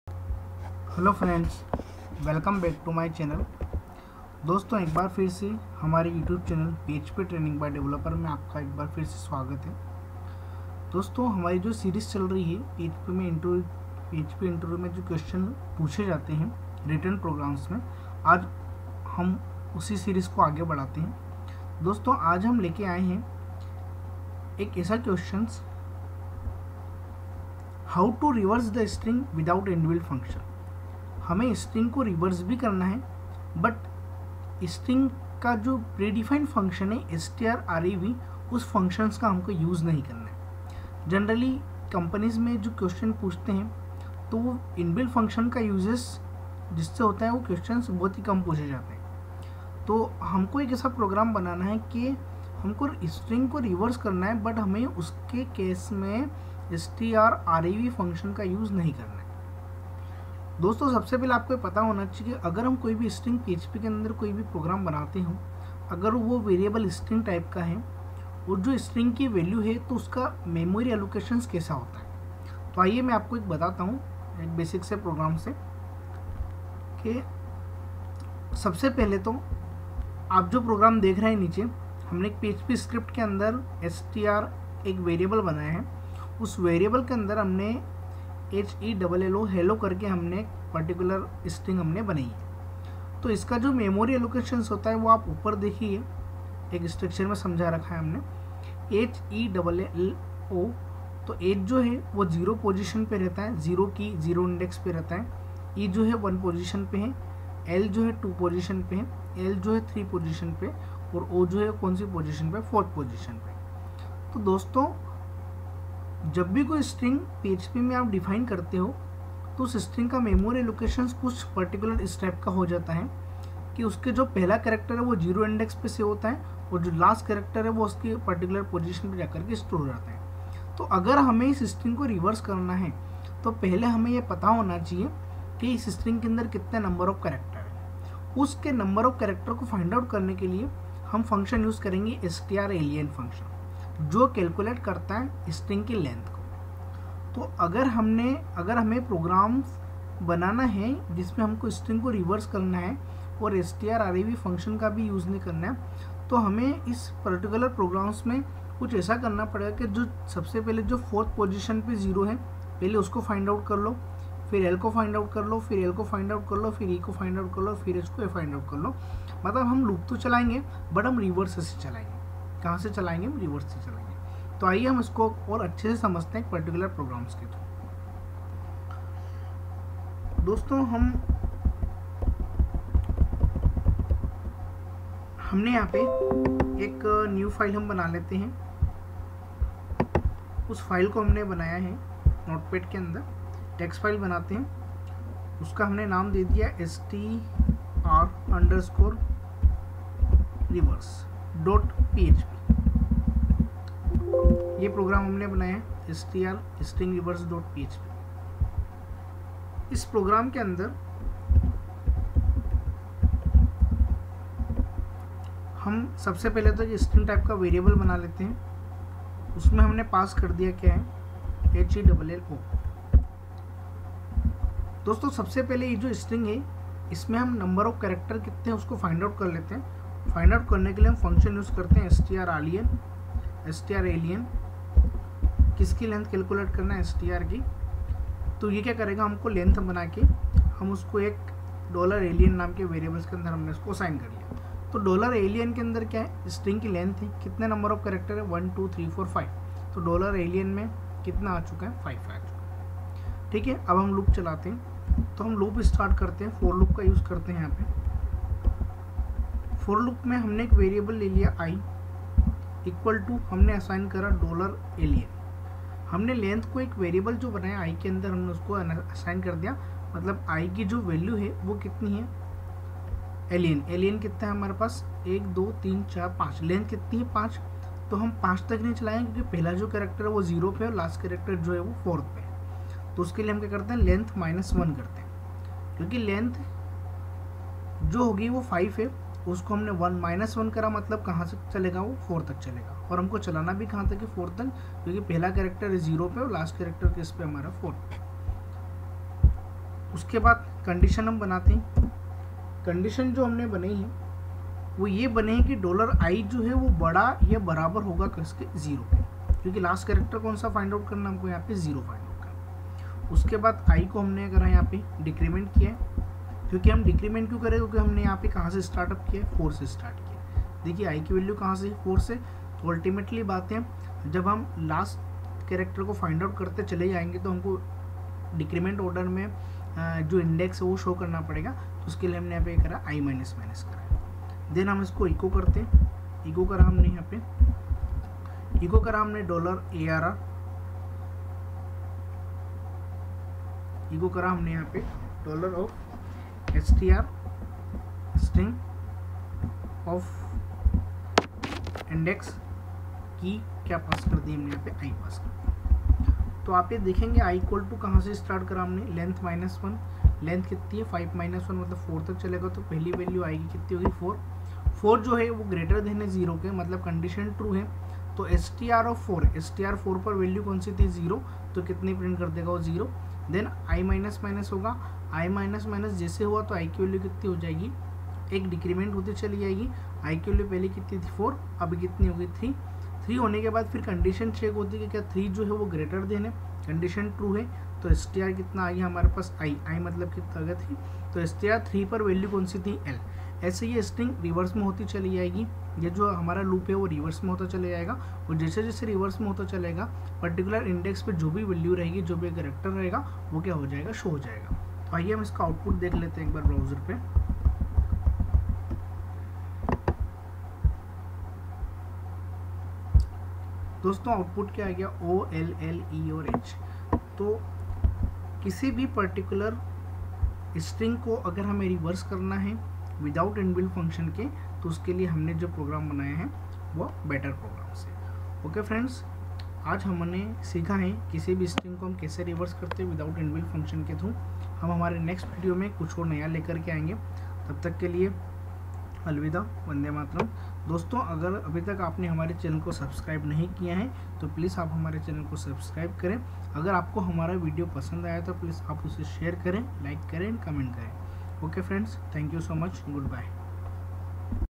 Reverse String without inbuilt function in PHP
In this video, Define how to reverse string without inbuilt function in php.